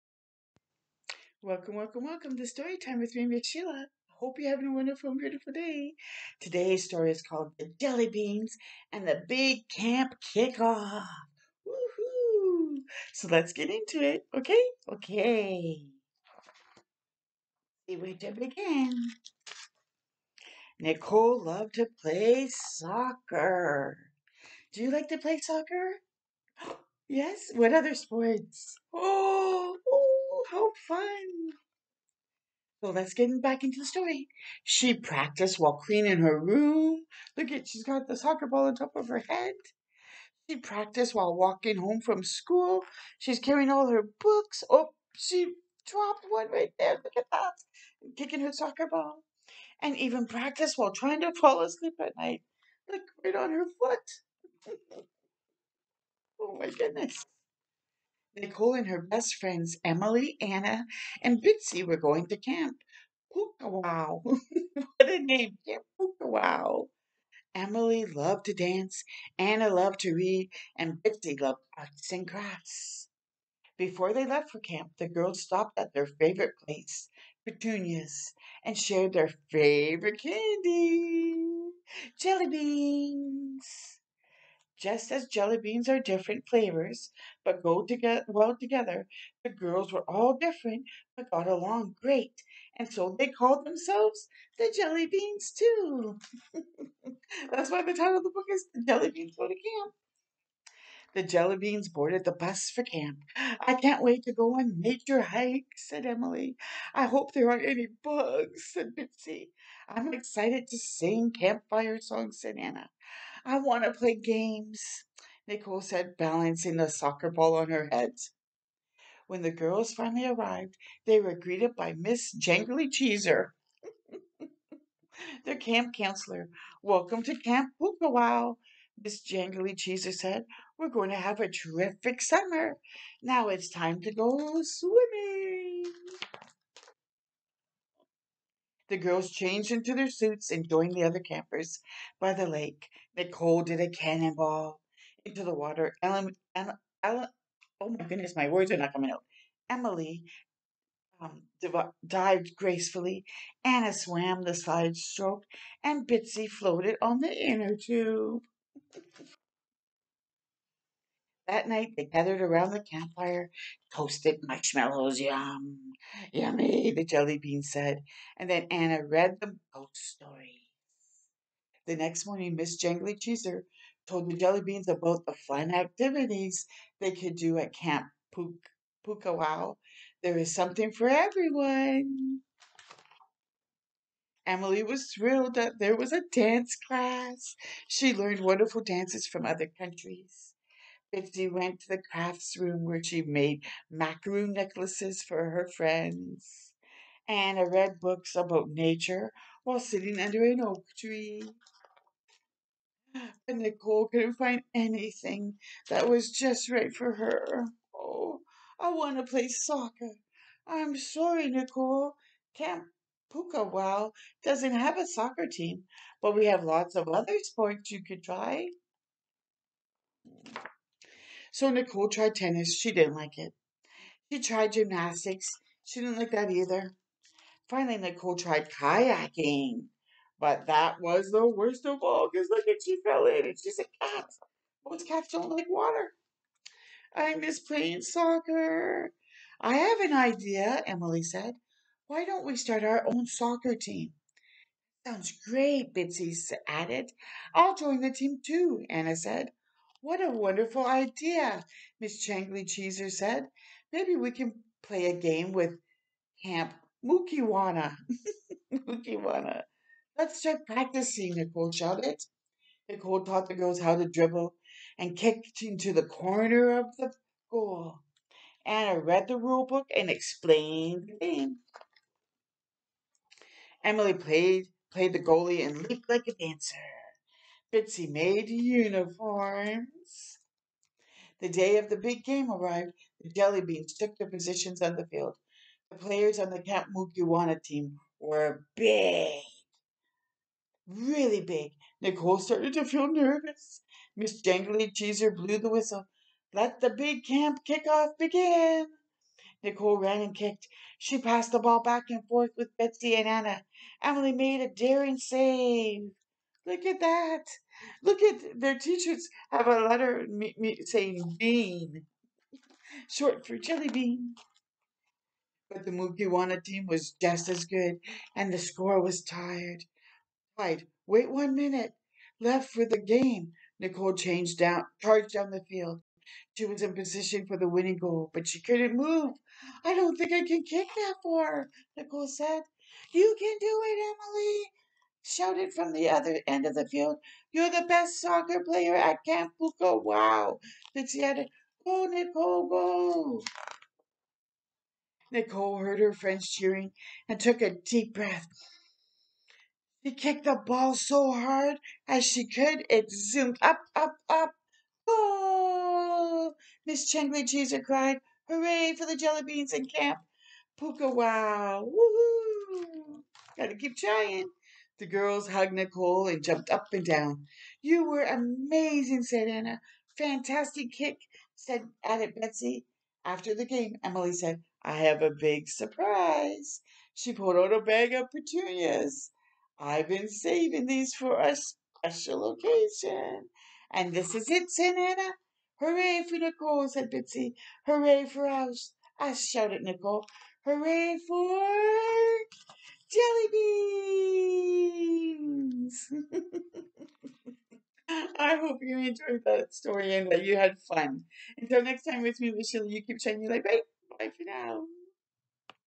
The Jellybeans & The Big Camp Kick Off Read Aloud
Laura Numeroff shares how just as jellybeans are all different flavors but go great together, friends can use their different strengths and talents to work as a team and have the best summer ever.